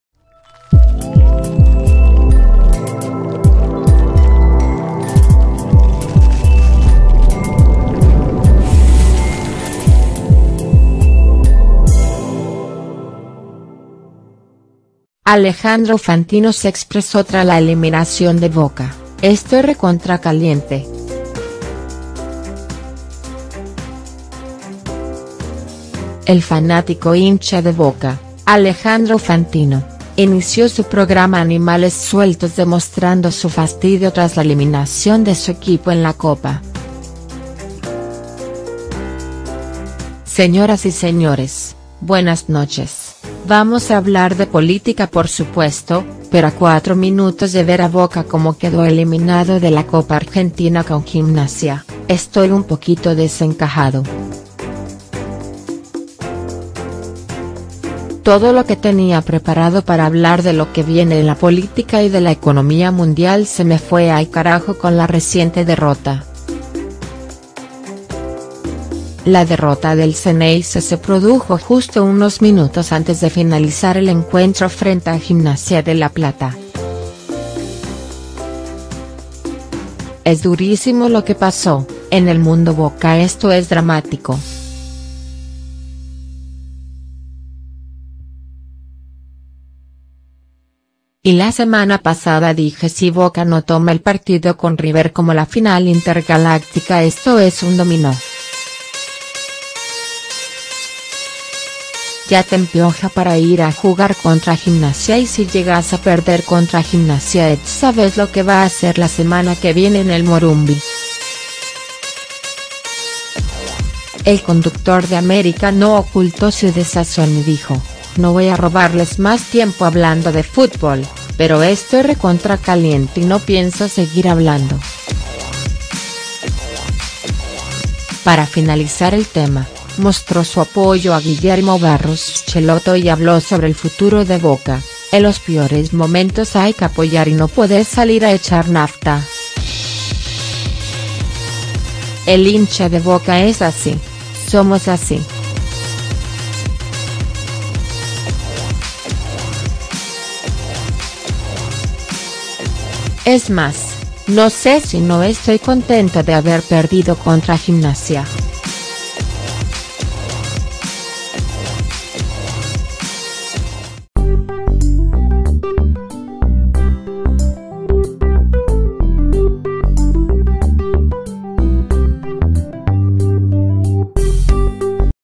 Alejandro Fantino se expresó tras la eliminación de Boca: "Estoy recontra caliente"
El conductor y locutor se mostró triste y enojado tras la eliminación de su equipo en la Copa Argentina